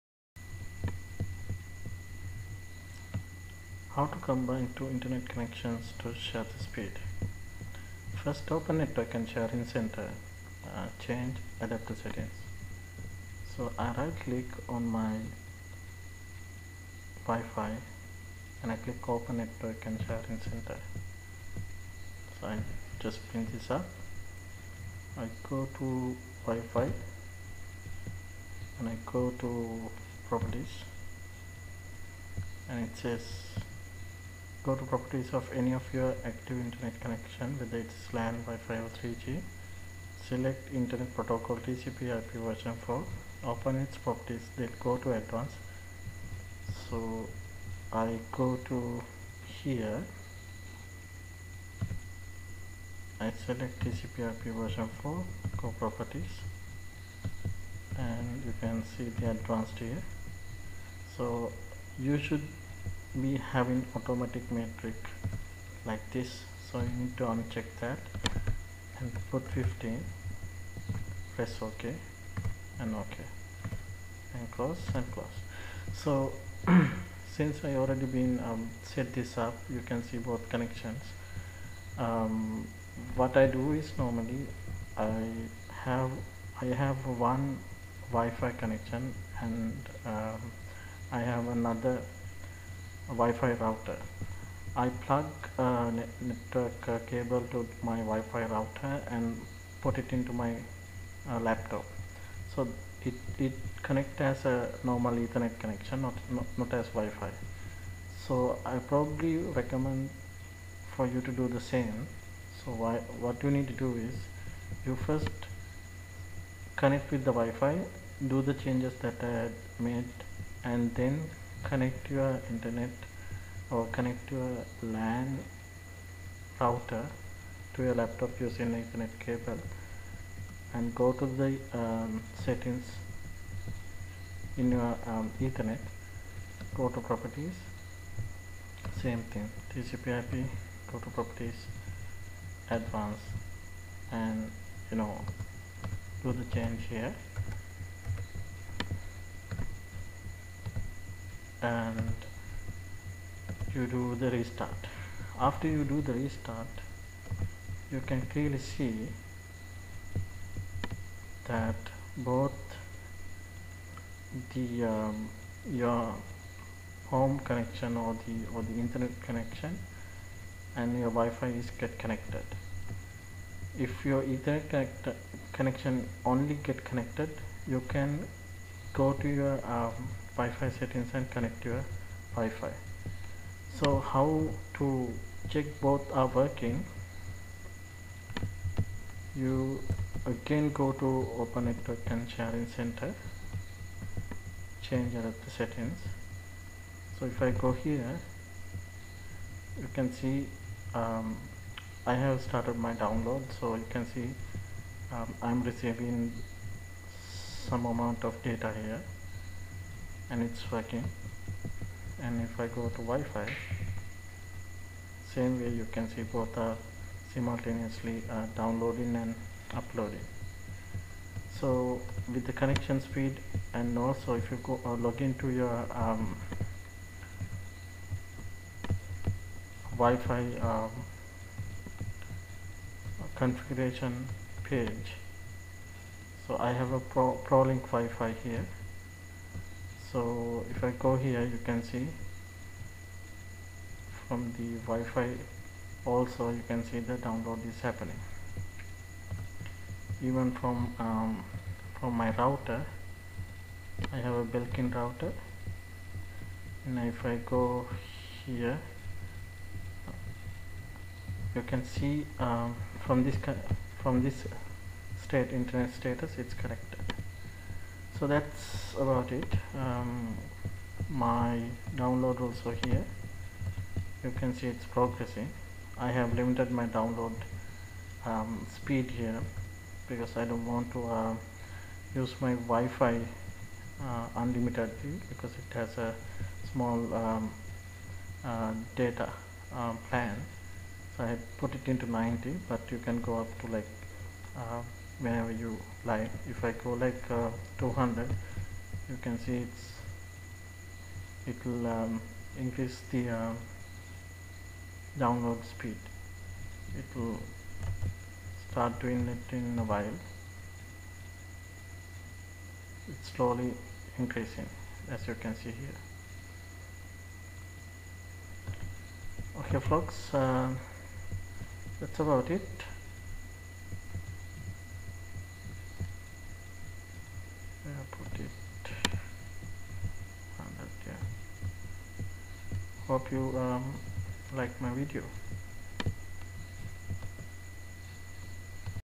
How to combine two internet connections
How I connect both my Ethernet and WiFi connection together so it's download/upload data simultaneously.